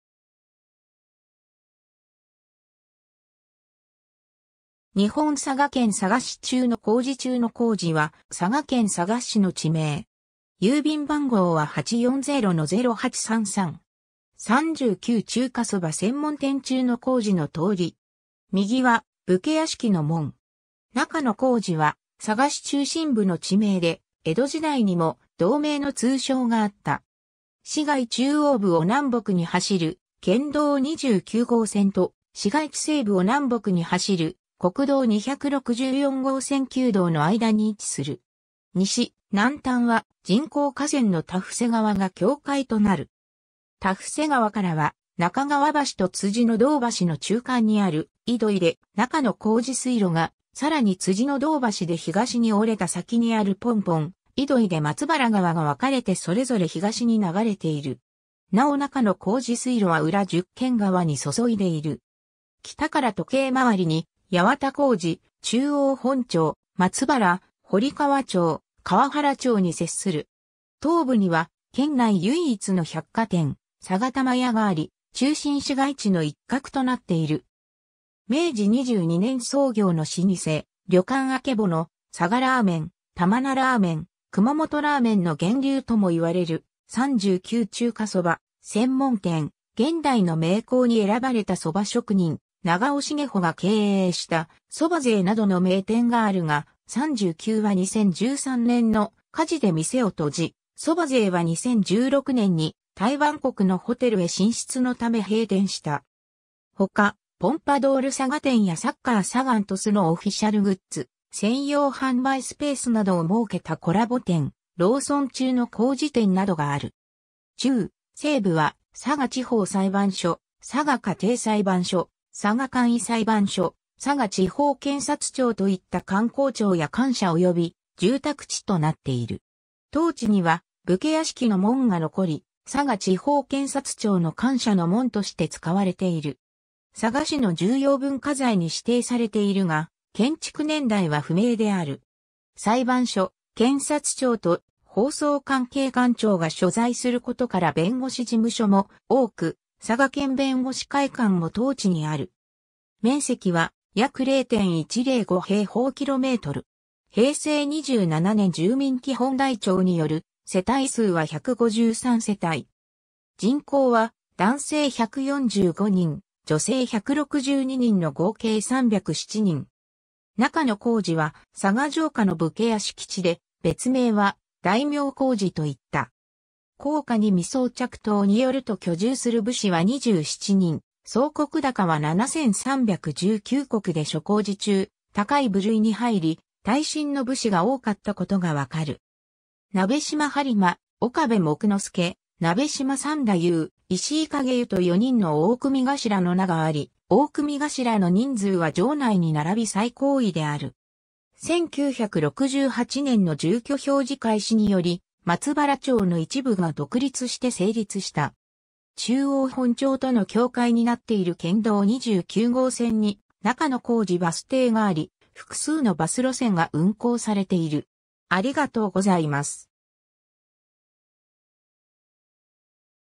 中の小路
佐賀市の町・字
日本 」 佐賀県 」 佐賀市 」 中の小路 中の小路（なかのこうじ）は佐賀県佐賀市の地名。
 三九中華そば専門店（2009年撮影） 中の小路の通り。
右は武家屋敷の門。
 中の小路は佐賀市中心部の地名で、江戸時代にも同名の通称があった。
市街中央部を南北に走る県道29号線（中央大通り）と市街地西部を南北に走る国道264号線旧道（紡績通り）の間に位置する。
西・南端は人工河川の多布施川が境界となる。
多布施川からは中川橋と辻の堂橋の中間にある井樋で中の小路水路が、さらに辻の堂橋で東に折れた先にあるポンポン井樋で松原川が別れてそれぞれ東に流れている。
なお中の小路水路は裏十間川に注いでいる。
北から時計回りに八幡小路、中央本町、松原、堀川町、川原町に接する。
 東部には県内唯一の百貨店「佐賀玉屋」があり、中心市街地の一角となっている。
明治22年創業の老舗「旅館あけぼの」、佐賀ラーメン・玉名ラーメン・熊本ラーメンの源流とも言われる「三九中華そば専門店」、現代の名工に選ばれたそば職人・長尾茂穂が経営した「そば勢」などの名店があるが、三九は2013年の火事で店を閉じ、そば勢は2016年にタイ・バンコクのホテルへ進出のため閉店した。
ほか、ポンパドウル佐賀店やサッカー・サガン鳥栖のオフィシャルグッズ専用販売スペースなどを設けたコラボ店、ローソン中の小路店などがある。
中・西部は佐賀地方裁判所・佐賀家庭裁判所・佐賀簡易裁判所・佐賀地方検察庁といった官公庁や官舎及び住宅地となっている。
当地には武家屋敷の門が残り、佐賀地方検察庁の官舎の門として使われている。
佐賀市の重要文化財に指定されているが建築年代は不明である。
裁判所・検察庁と法曹関係官庁が所在することから弁護士事務所も多く、佐賀県弁護士会館も当地にある。
 面積は約0.105㎢。
平成27年住民基本台帳による世帯数は153世帯。
人口は男性145人・女性162人の合計307人。
 中の小路は佐賀城下の武家屋敷地で別名は大名小路といった。
『弘化二巳総着到』（鍋島報效会所蔵の佐賀藩士名簿）によると居住する武士は27人、総石高は7,319石（平均271石）で諸小路中高い部類に入り、大身の武士が多かったことがわかる。
鍋島播磨、岡部杢之助、鍋島三太夫、石井勘解由と4人の大組頭の名があり、大組頭の人数は城内に並び最高位である。
 1968年（昭和43年）の住居表示開始により松原町の一部が独立して成立した。
 中央本町との境界になっている県道29号線に「中の小路」バス停があり、複数のバス路線が運行されている。